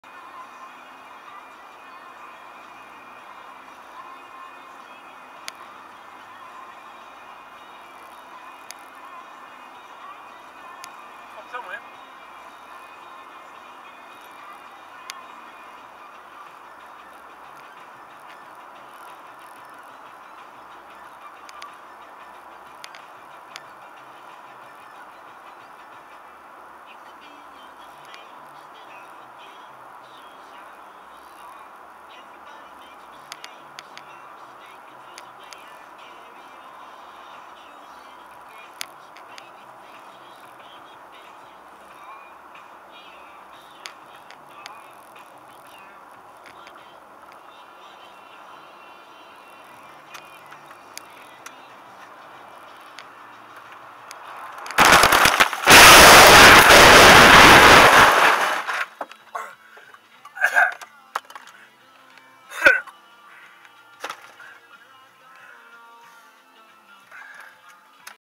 My Crash
Slid on Black Ice and then hit a bridge